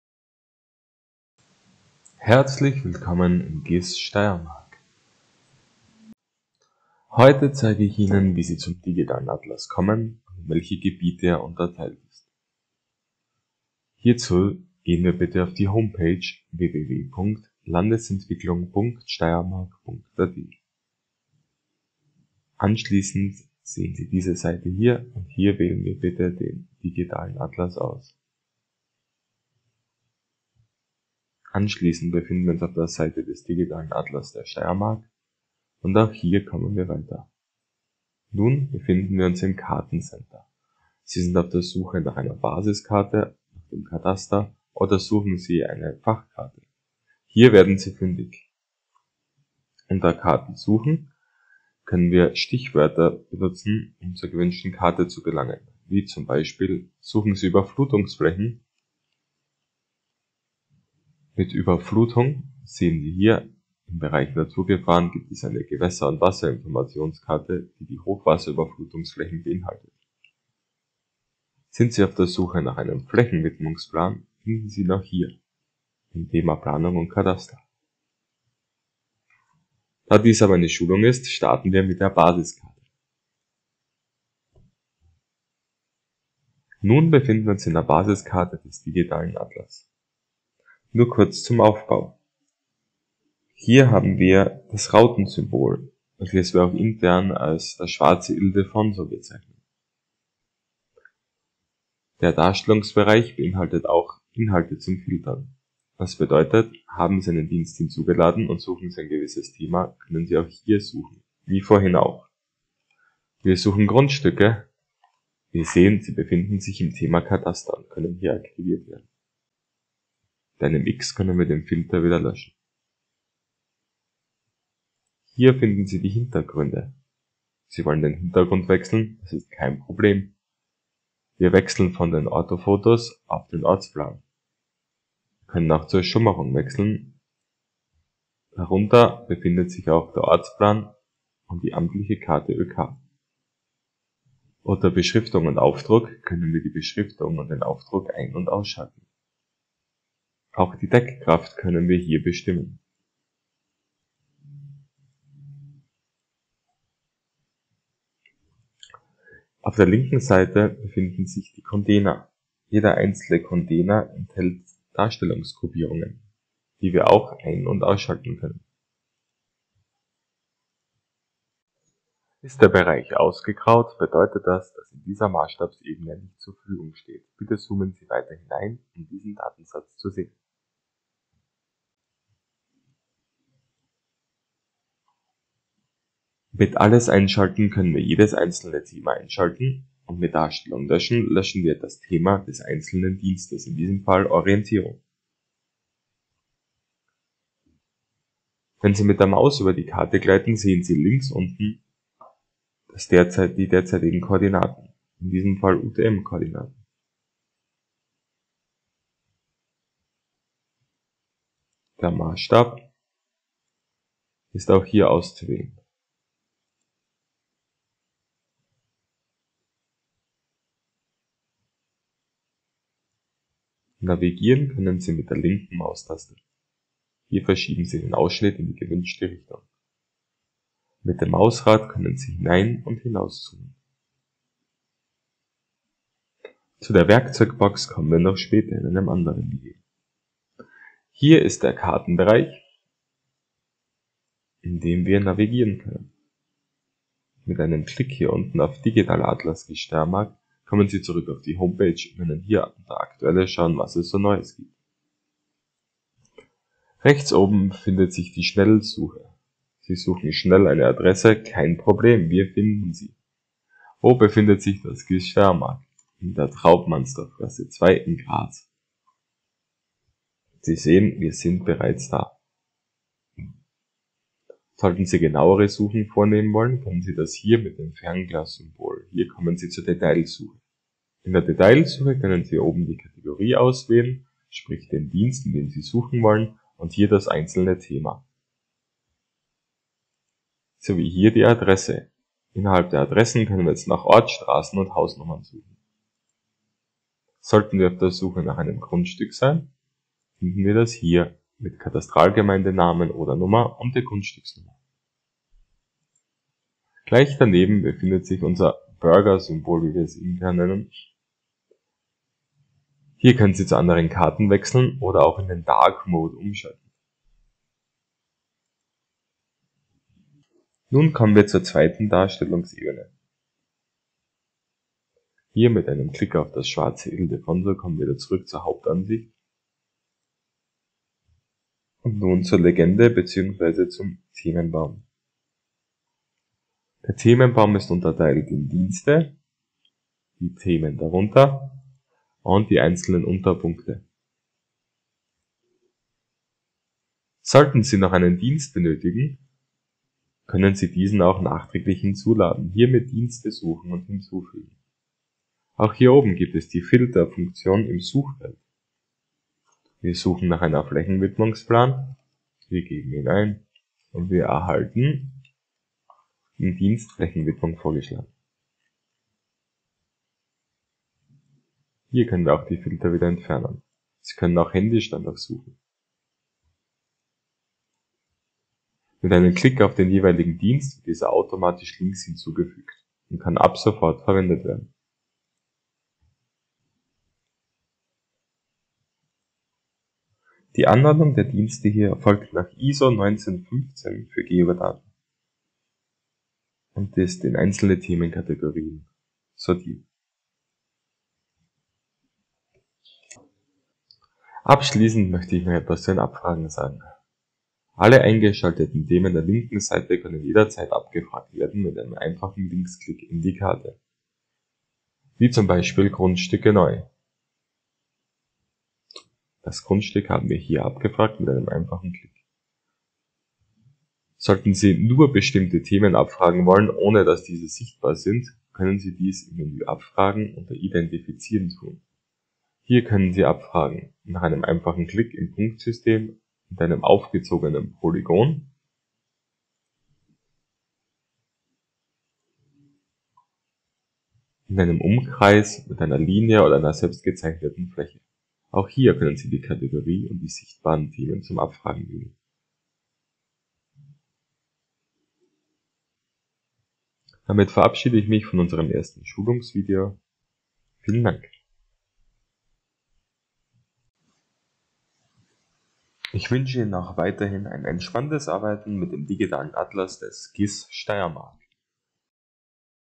GIS-Steiermark – Grundlagen WebGIS
Mit dieser Videoreihe versuchen wir die Online-Hilfe zu ergänzen, um Ihnen das Arbeiten etwas zu erleichtern und neuen Usern einen einfacheren Start ins Webgis des GIS-Steiermark (Digitaler Atlas/webGIS pro) zu ermöglichen.